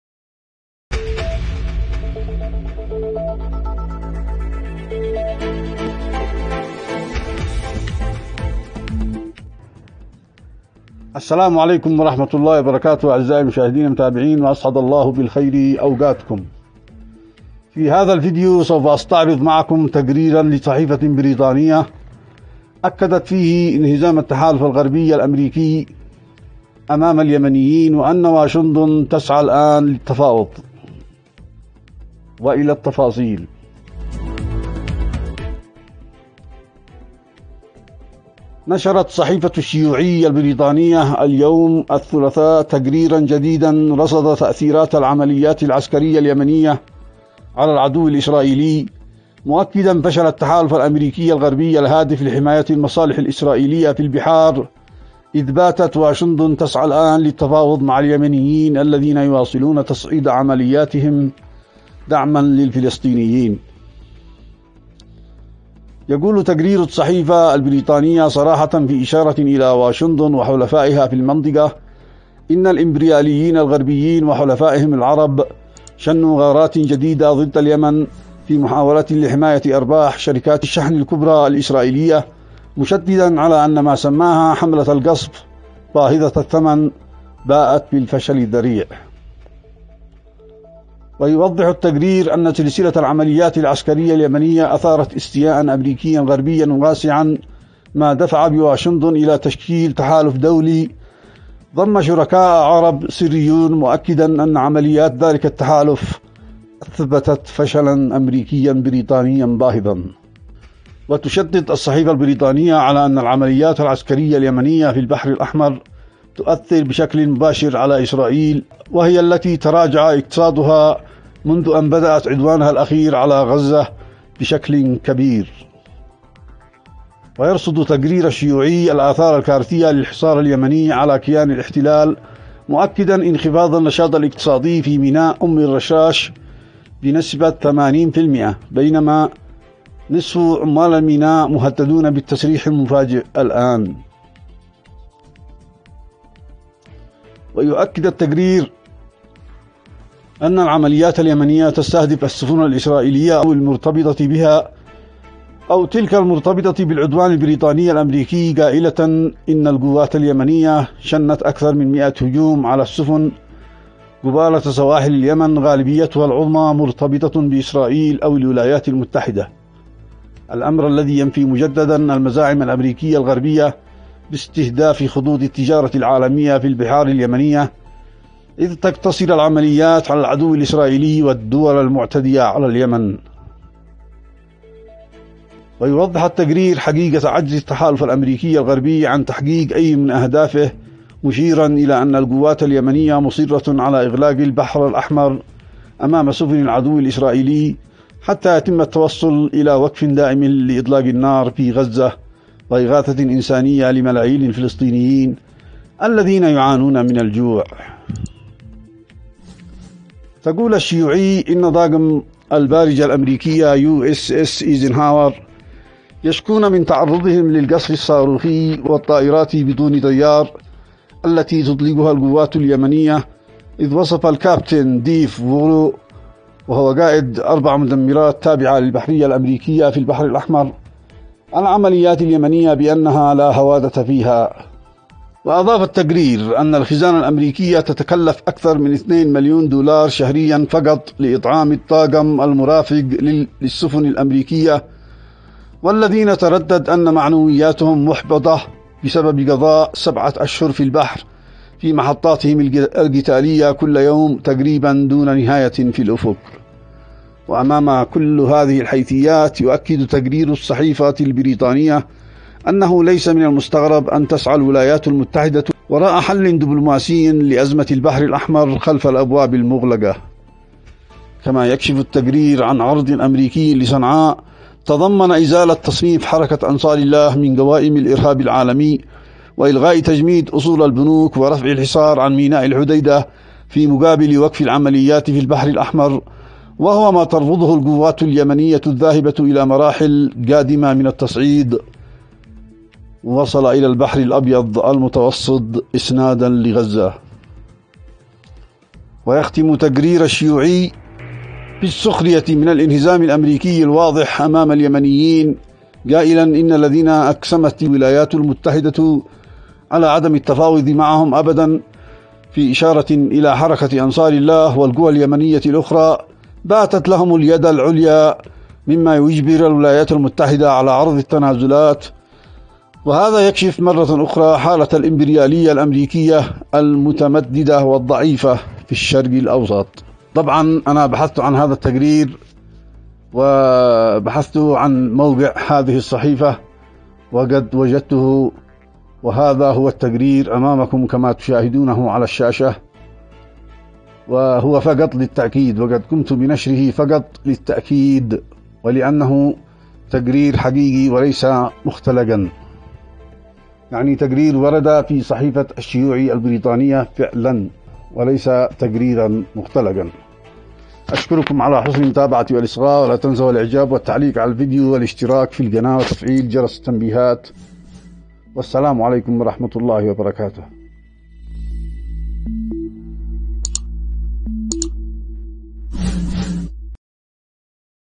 صحيفة بريطانية: التحالف الأمريكي الغربي خسر أمام اليمنيين وواشنطن تسعى الآن للتفاوض🙏
صحيفة بريطانية: التحالف الأمريكي الغربي انهزم أمام اليمنيين وواشنطن تسعى الآن للتفاوض🙏

مسلسل ماء الذهب
اشتباكات_ البحر_الأحمر
مواجهات_ البحر_الأحمر